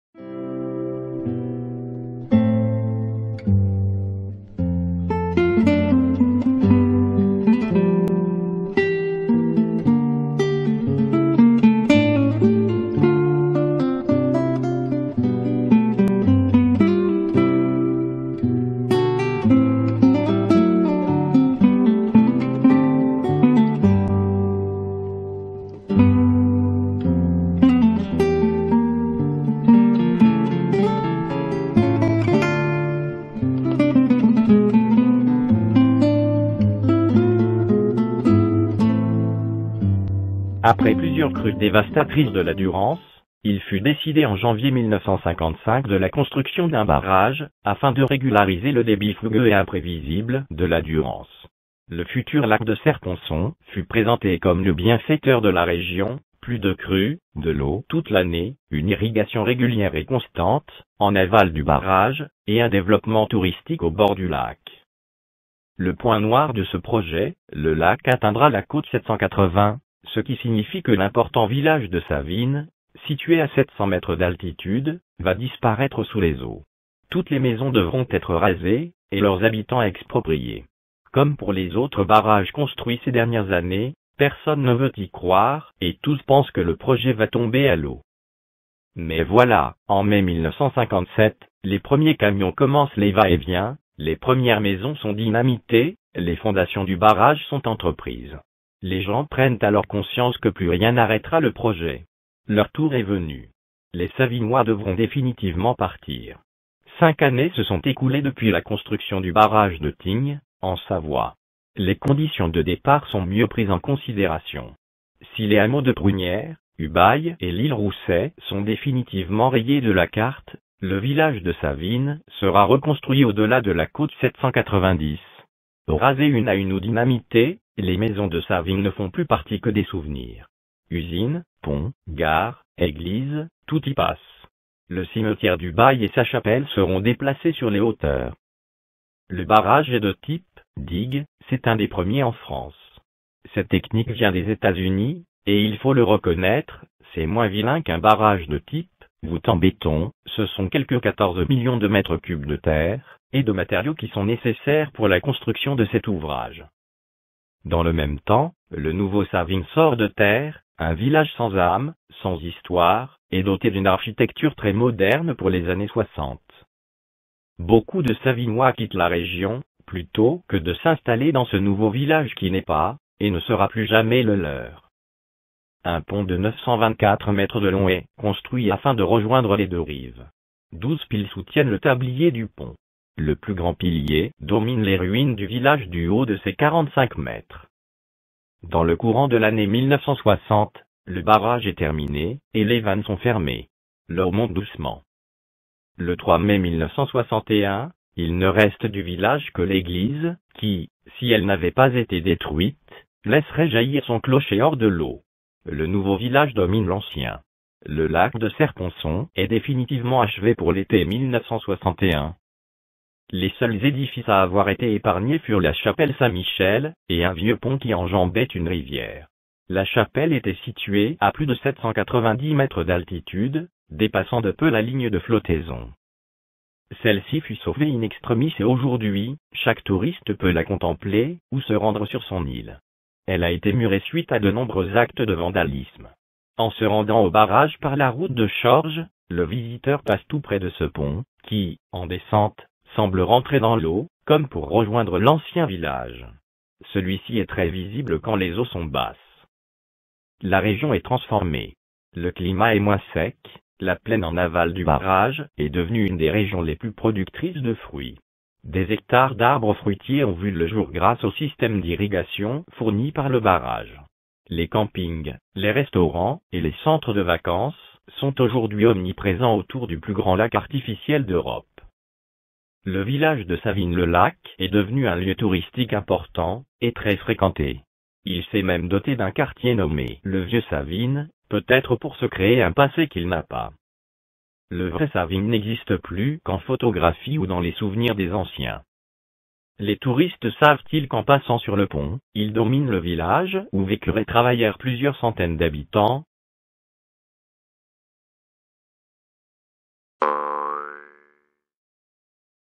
SAVINES ET LE BARRAGE DE SERRE-PONCON
Après plusieurs crues dévastatrices de la Durance, il fut décidé en Janvier-1955 de la construction d'un barrage, afin de régulariser le débit fougueux et imprévisible de ce fleuve. Le futur lac de Serre-Ponçon fut présenté comme le bienfaiteur de la région : plu de crues, de l'eau toute l'année, une irrigation régulière et constante en aval du barrage et  un développement touristique au bord du lac.

Le point noir de ce projet : le lac atteindra la côte 780, ce qui signifie que l'important village de Savines, situé à 700 mètres d'altitude, va disparaître sous les eaux. Toutes les maisons devront être rasées, et leurs habitants expropriés. Comme pour les autres barrages construits ces dernières années, personne ne veut y croire et tous pensent que le projet va tomber à l'eau.

Mais voilà, en mai 1957, les premiers camions commencent les va-et-vient, les premières maisons sont dynamitées, les fondations du barrage sont entreprises. Les gens prennent alors conscience que plu rien n'arrêtera le projet. Leur tour est venu. Les Savinois devront définitivement partir. 5 années se sont écoulées depuis la construction du barrage de Tignes, en Savoie. Les conditions de départ sont mieux prises en considération. Si les hameaux de Prunières, Ubaye et l'Île Rousset sont définitivement rayés de la carte, le village de Savines sera reconstruit au delà de la côte 790. Rasées une à une ou dynamitées, les maisons de Savines ne font plus parties que des souvenirs. Usines, pont, gare, Eglise, tout y passe. Le cimetière de Ubaye et sa chapelle seront déplacés sur les hauteurs.

Le barrage est de type "digue". C'est un des premiers en France. Cette technique vient des Etats Unis, et il faut le reconnaître, c'est moins vilain qu'un barrage de type "voûte en béton". Ce sont quelques 14 millions de mètres-cube de terre et de materiaux qui sont nécessaires pour la construction de cet ouvrage.

Dans le même temps, le nouveau Savines sort de terre: un village sans âme, sans histoire et doté d'une architecture très moderne pour les années 60.

Beaucoup de Savinois quittent la région, plutôt que de s'installer dans ce nouveau village qui n'est pas, et ne sera plus jamais le leur.

Un pont de 924 mètres de long haie construit afin de rejoindre les deux rives. 12 piles soutiennent le tablier du pont. Le plus grand pilier domine les ruines du village du haut de ses 45 mètres.

Dans le courant de l'année 1960, le barrage est terminé et les vannes sont fermées. L'eau monte doucement.

Le 3 mai 1961, il ne reste du village que l'Eglise qui, si elle n'avait pas été détruite, laisserait jaillir son clocher hors de l'eau. Le nouveau village domine l'ancien. Le lac de Serre-Ponçon est définitivement achevé pour l'été 1961.

Les seuls édifices à avoir été épargnés furent la chapelle-Saint Michel, et un vieux pont qui enjambait une rivière. La chapelle était située à plus de 790 mètres d'altitude, dépassant de peu la ligne de flottaison.

Celle-ci fut sauvée in-extremiss et aujourd'hui, chaque touriste peut la contempler ou se rendre sur son île.
 Elle a été murée suite à de nombreux actes de vandalisme. En se rendant au barrage par la route de Chorges, le visiteur passe tout près de ce pont qui, en descente, semble rentrer dans l'eau comme pour rejoindre l'ancien village. Celui-ci est très visible quand les eaux sont basses.

La région est transformée. Le climat est moins sec, la plaine en aval du barrage est devenue une des régions les plus productrices de fruits. Des hectares d'arbres fruitiers ont vu le jour grâce au système d'irrigation fournit par le barrage. Les campinngs, les restaurant et les centres de vacances sont aujourd'hui omniprésents autour du plus grand lac artificiel d'Europe.

Le village de Savines-le-lac est devenu un lieu touristique important et très fréquenté. Il sait même dôté d'un quartier nommé le vieux Savines, peut être pour se créer un passé qu'il n'a pas.

Le vrai Savines n'existe plus qu'en photographies ou dans les souvenirs des anciens.

Les touristes savent-ils qu'en passant sur le pont, ils dominent le village où vécurent et travaillèrent plusieurs centaines d'habitants?